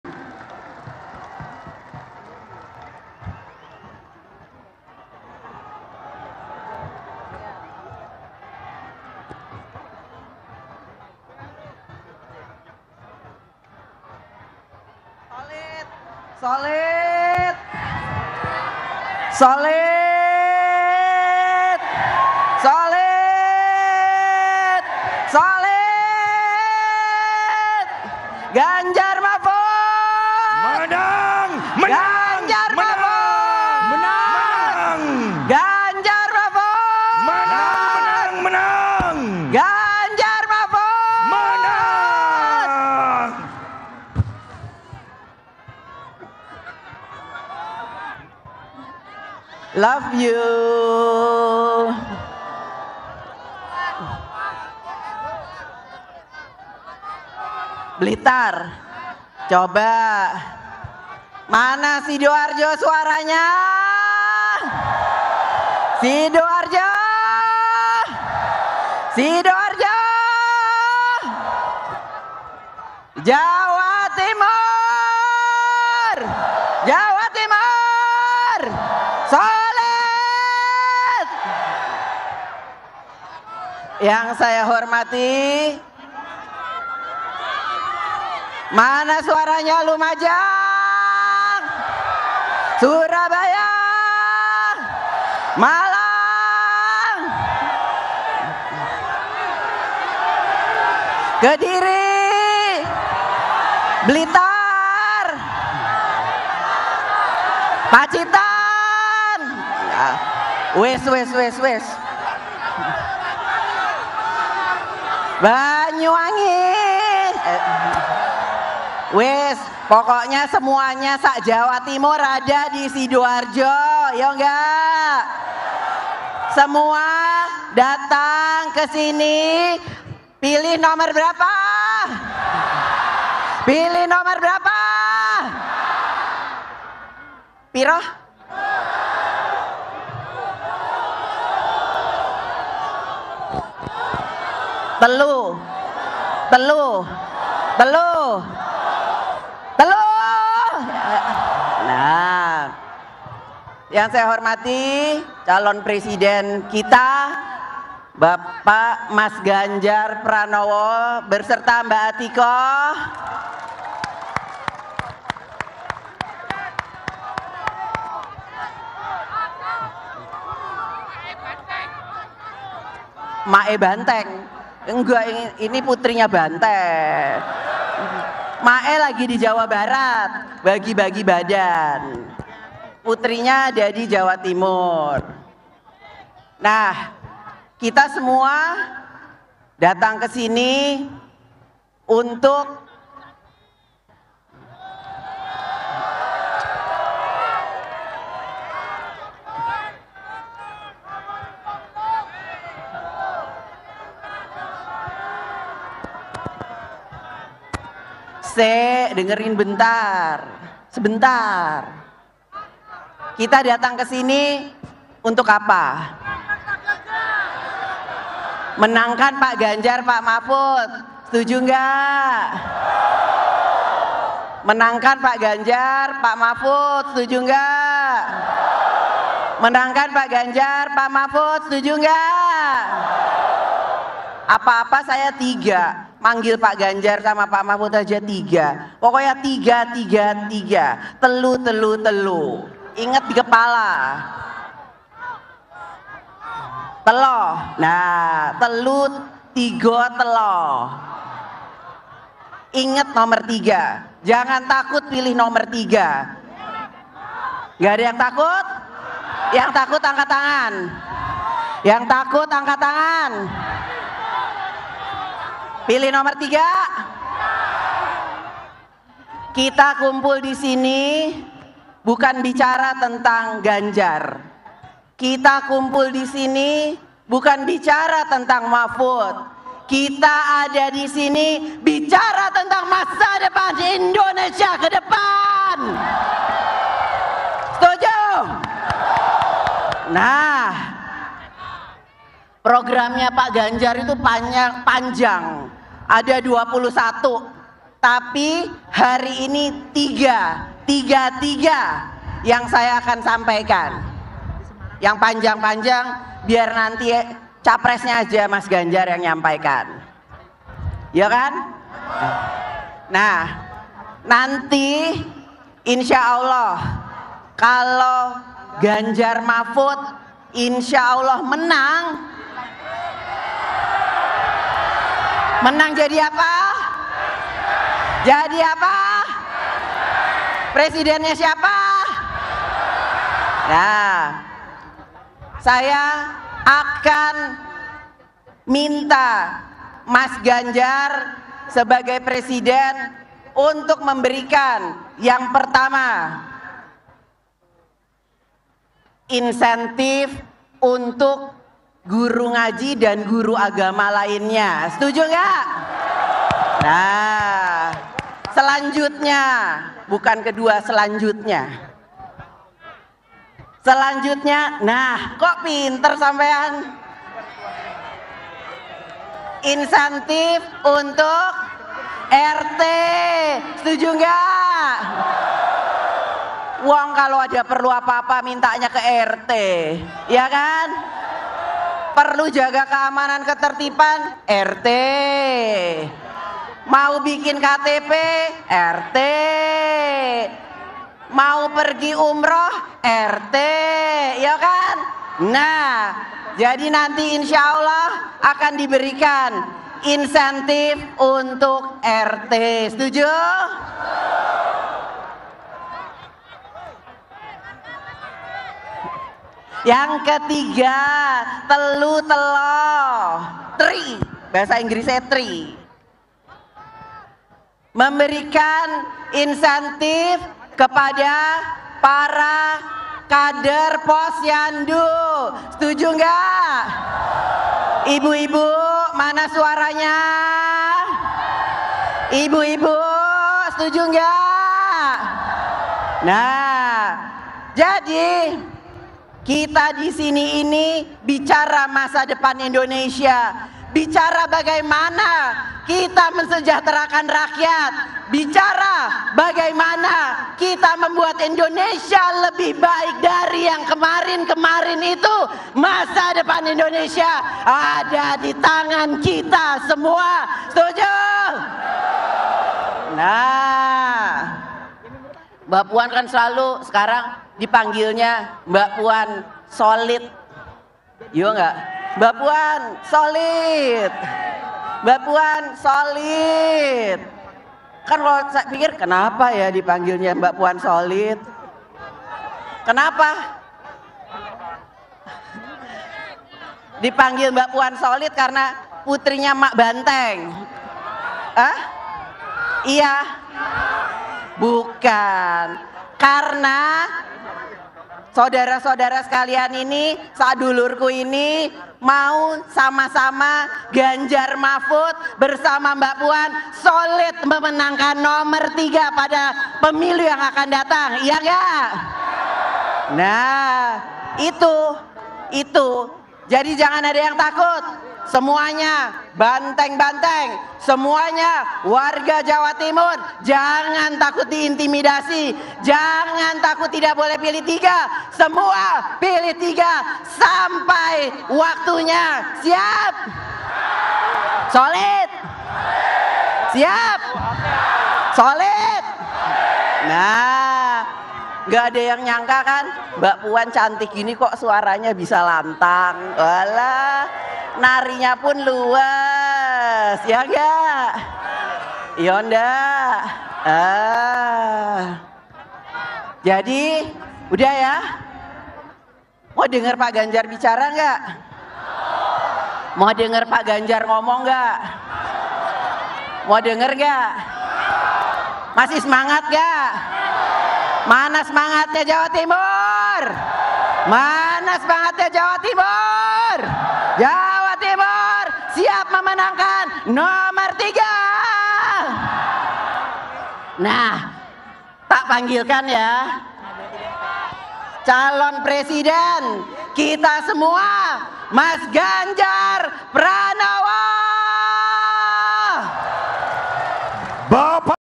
Kampanye Akbar Puan Maharani di Jawa Timur: Solid Menangkan Ganjar-Mahfud Satu Putaran
KOMPAS.TV - Kampanye Akbar Puan Maharani  di Jawa Timur: Solid Menangkan Ganjar-Mahfud Satu Putaran